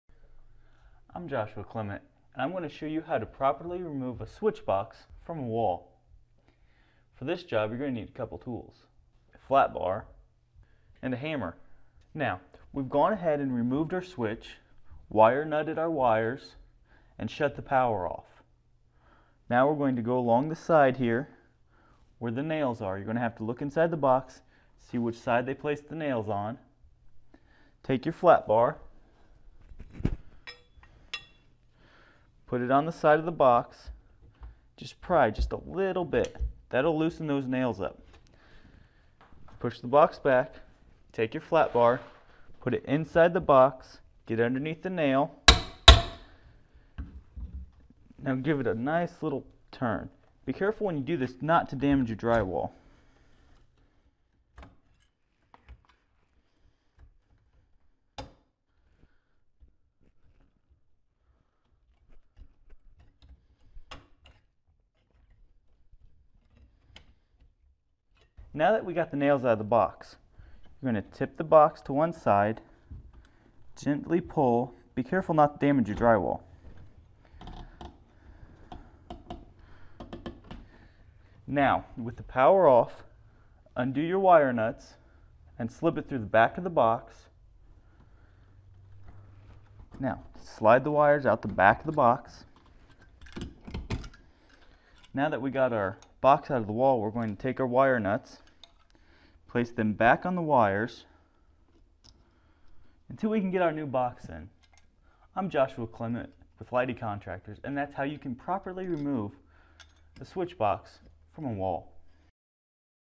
How to Remove a Switch Box From a Wall
How to Remove a Switch Box From a Wall. Part of the series: All About Walls. Removing a switch box from the wall must be done properly to avoid damaging the box and the wall itself. Remove a switch box from a wall with help from a foreman and contractor in this free video clip.